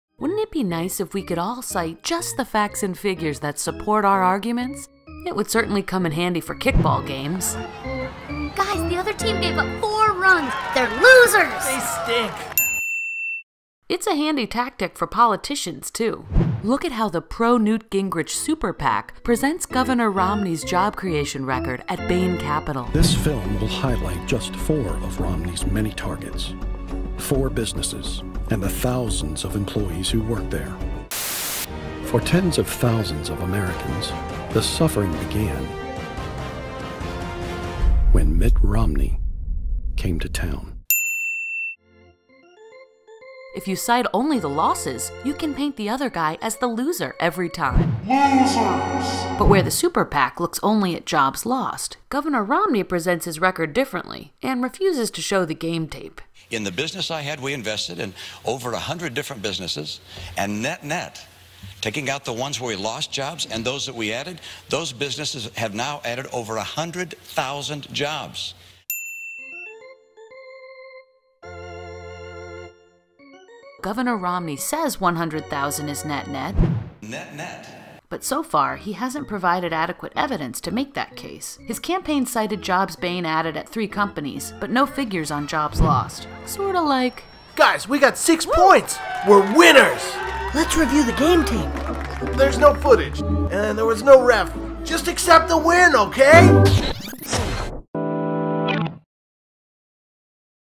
Evidence and its absence about the Romney record on jobs.
Flackcheck.org applies the jobs logic of the pro Gingrich Super Pac and Governor Romney to scoring kickball games.